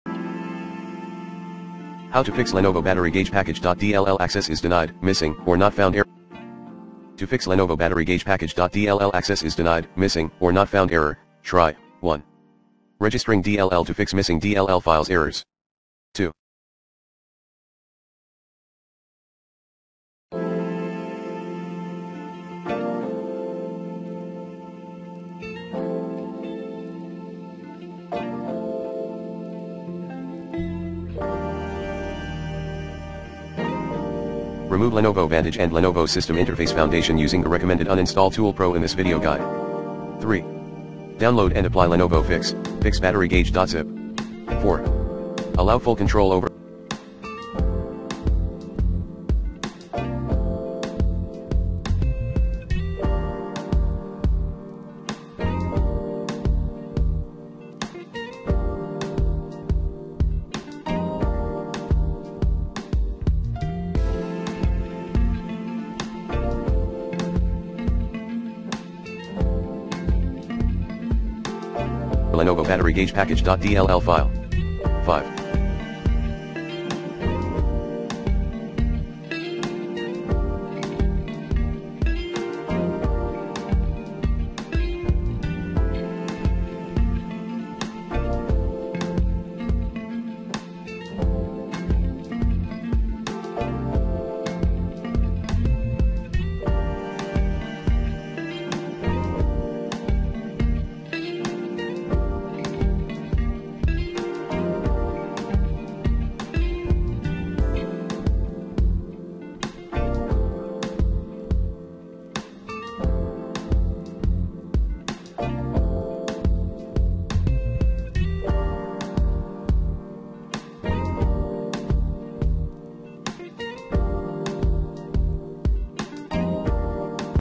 Fix LenovoBatteryGaugePackage.dll access is denied, missing or not found error on Windows 10
Here's how to Fix LenovoBatteryGaugePackage.dll access is denied, missing or not found error on Windows 10. To effectively remove unwanted/problematic programs, run this best uninstall tool@